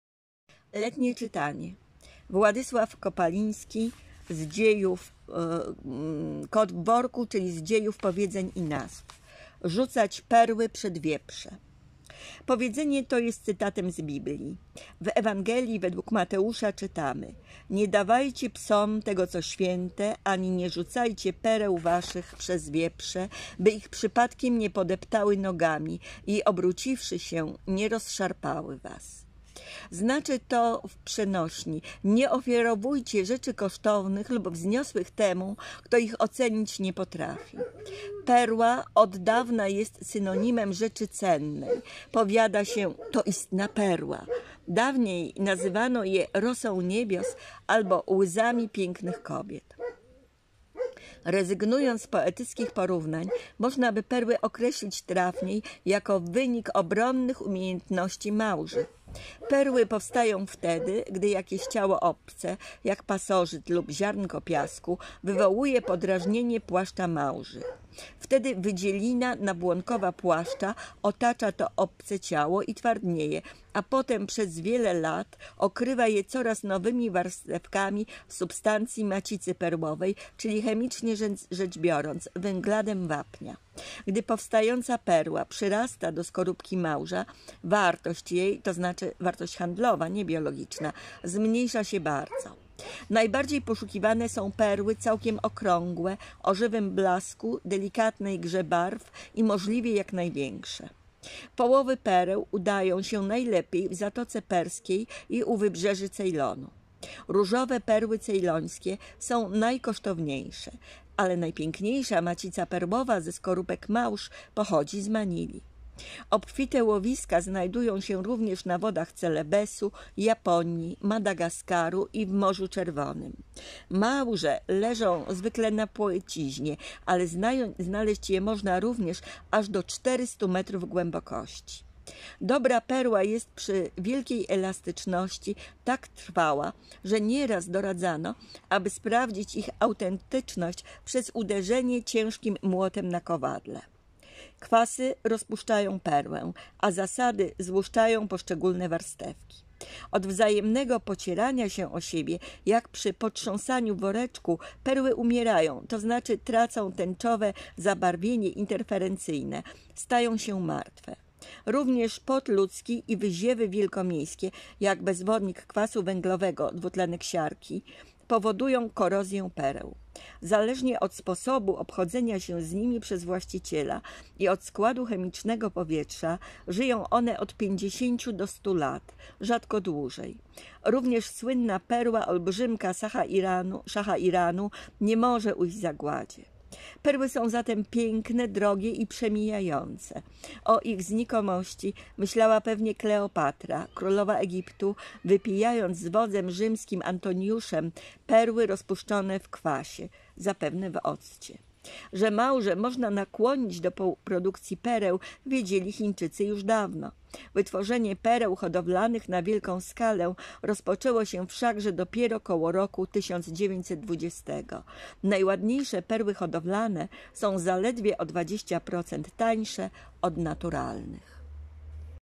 Letnie czytanie: Rzucać perly przed wieprze
Władysław Kopaliński "Kot w worku czyli z dziejów powiedzeń i nazw" Krajowa Agencja Wydawnicza 1977